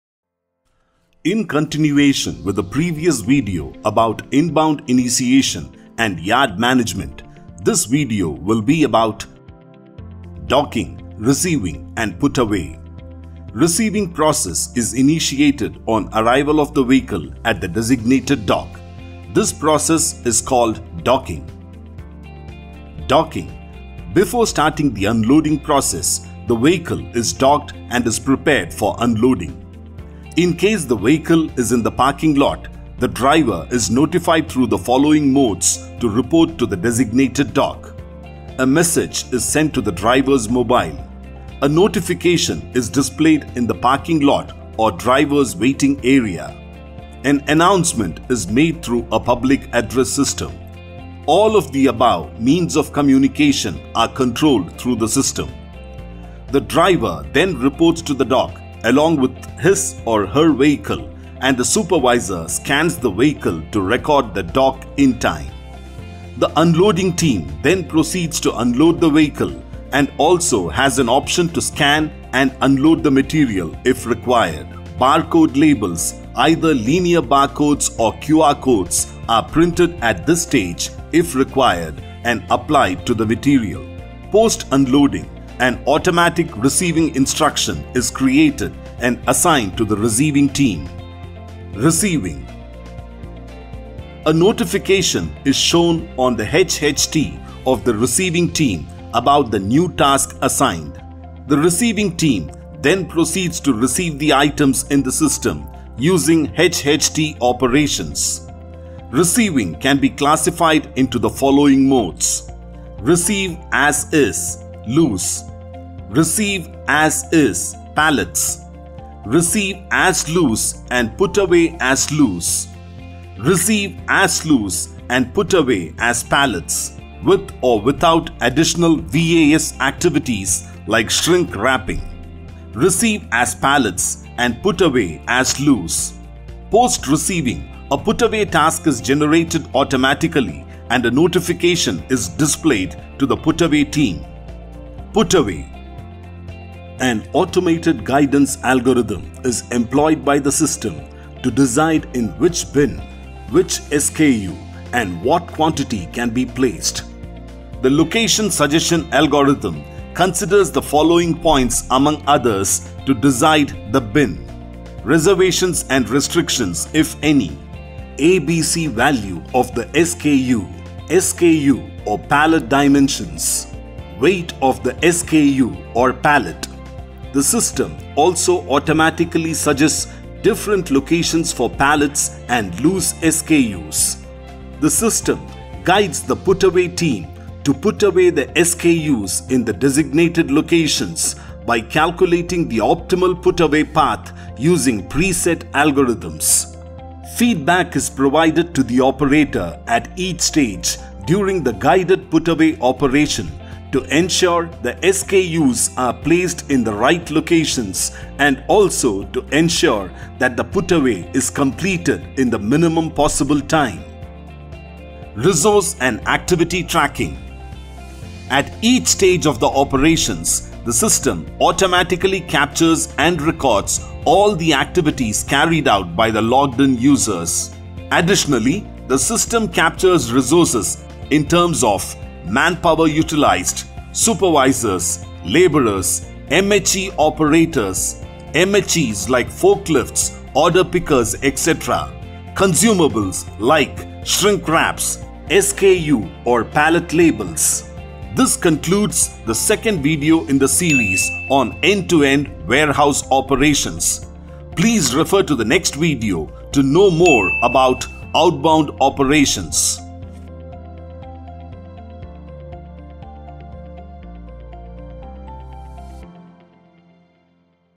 Receiving and Put Away Process in Warehouse Operations | WMS
This video will be about Receiving and Put Away Process in Warehouse Operations also docking, uploading goods receiving them and Labelling. Put-away System automatically decides in which bin, what SKU and quantity the good are to be put. Resource & activity tracking in Warehouse Management.